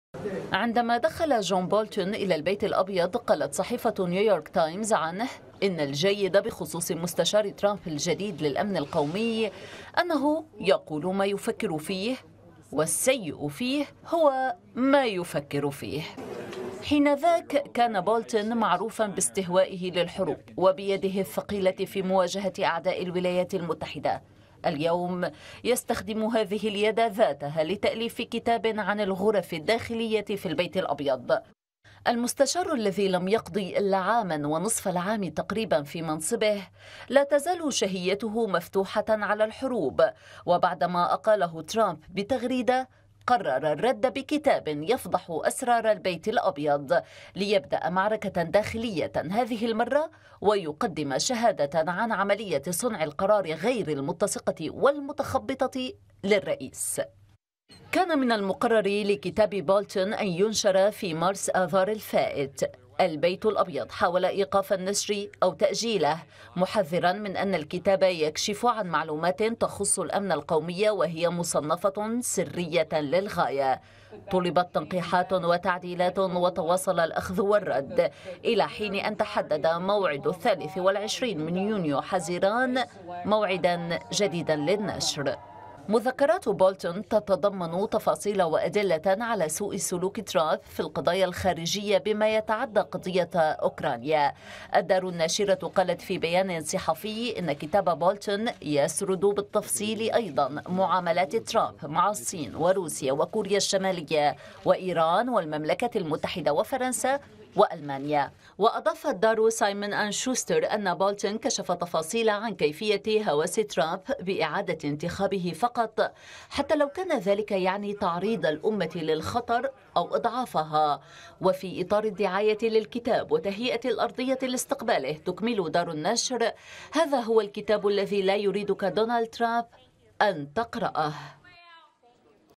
بولتون يعتزم نشر كتاب يكشف فيه تجاوزات ترامب 13‏/06‏/2020 أعلنت دار نشر أميركية أن مستشار الأمن ال
بولتون يعتزم نشر كتاب يكشف فيه تجاوزات ترامب
 13‏/06‏/2020

أعلنت دار نشر أميركية أن مستشار الأمن القومي السابق جون بولتون سينشر كتاب مذكرات يكشف فيه تجاوزات ارتكبها ترامب رغم تحذير الأخير سابقاً من نشره.
ولفت الناشر في بيان إلى أن بولتون سيوثق مخالفات ارتكبها ترامب تتعدى الضغوط التي مارسها على أوكرانيا للتحقيق مع منافسه الديموقراطي جو بايدن وأدت إلى اتهامه ومحاكمته في الكونغرس، مشيراً إلى أن بولتون سيصف في مذكراته عملية اتخاذ القرار "المتناقضة والمشتتة" لترامب.